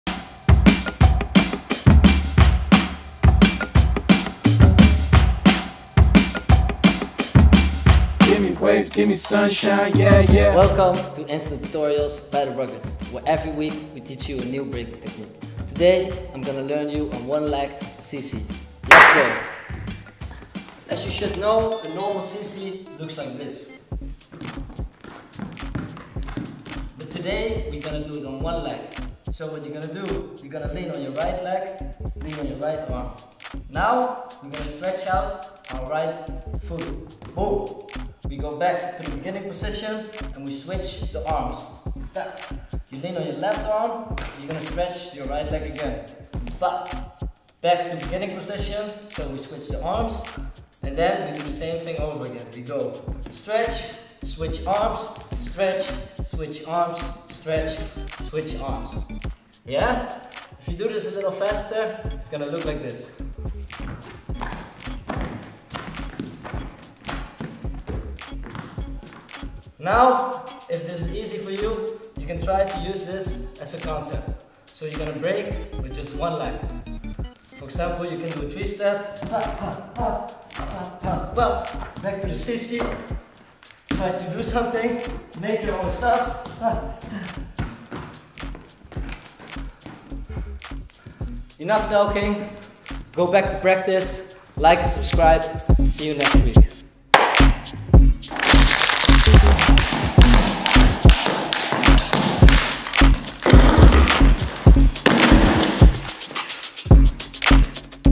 IMPROVE YOUR FOOTWORK // ONE LEGGED CC // INSTANT TUTORIAL // BREAKING BASICS
This week we have ONE LEGGED CC on the menu. To improve your FOOTWORK you can give yourself some rules or better said 'BREAK THE RULES'.

As you might know the CC is one of the basics in footwork and we use it to transition into a different step or we use it to go into a different direction. Some people use it to fill up some time when they don't know what to do, but that's not what we want you to do so.. Stepper made this concept for you to play around with and switch up your old CC habits.

It takes time to build a vocabulary, but these are definitely tools you should work with.

Teacher: @steppertheruggeds
Video: @freddydojo